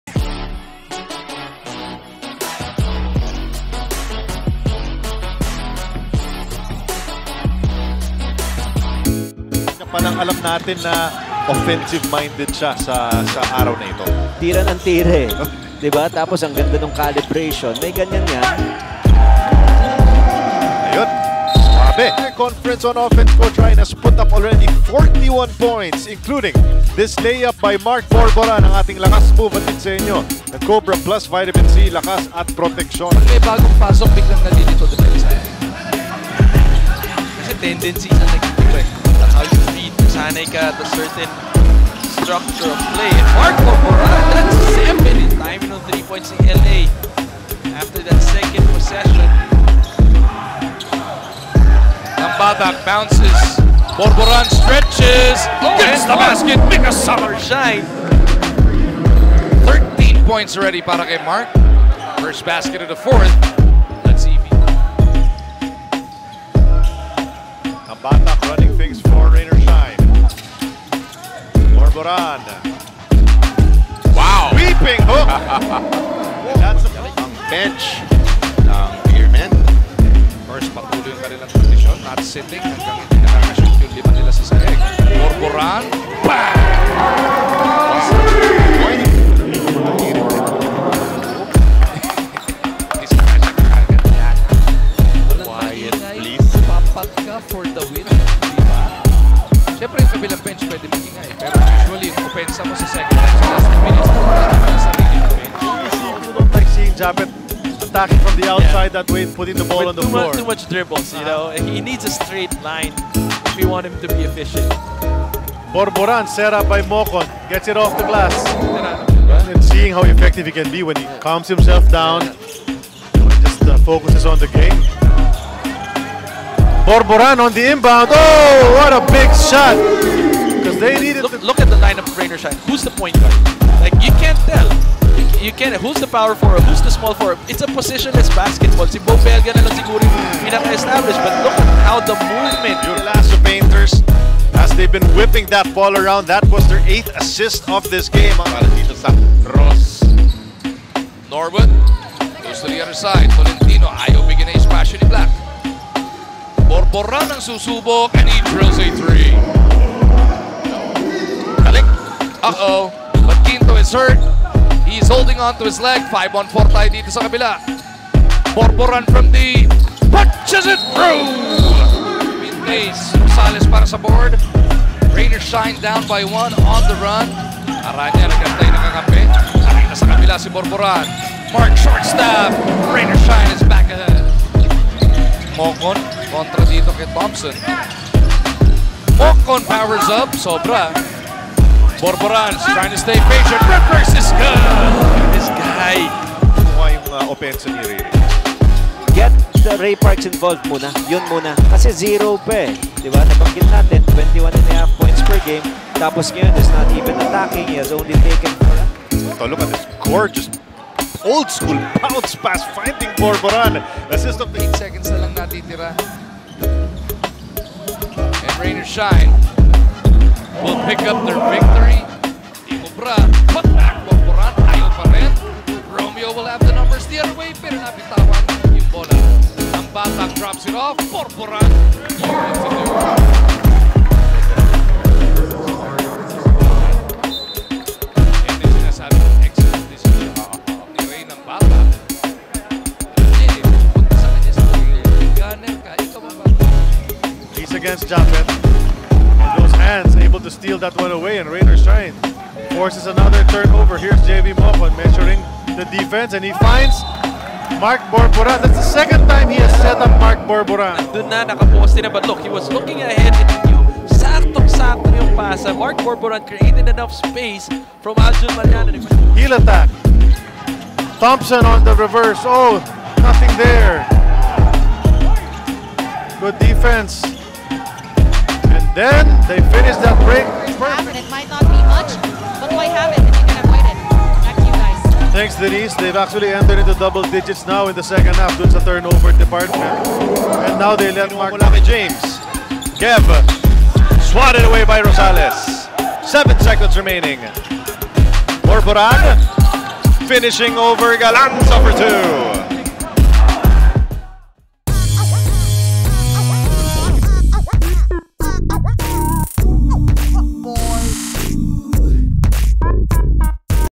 Mark Borboran ROS 2020 PH CUP Highlights
A role playing wingman who has been touted for his ability to pass, rebound and defend while being a constant threat in the perimeter.